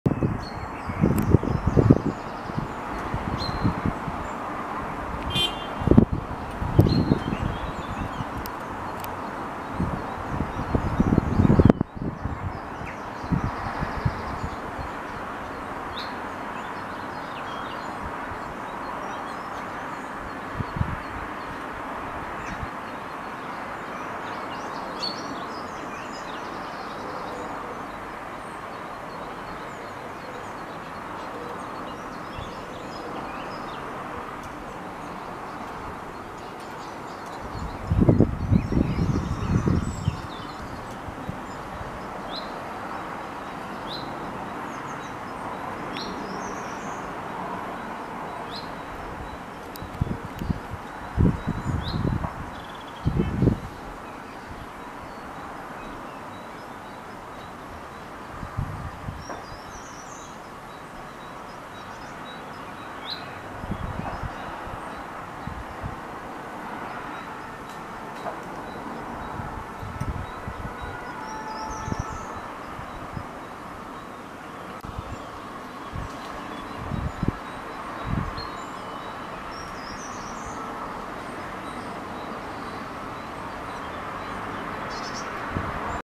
Merle noir femelle qui prend un bain de soleil
Un merle noir femelle prend un bain de soleil.
In the video you can see a female blackbird sunbathing.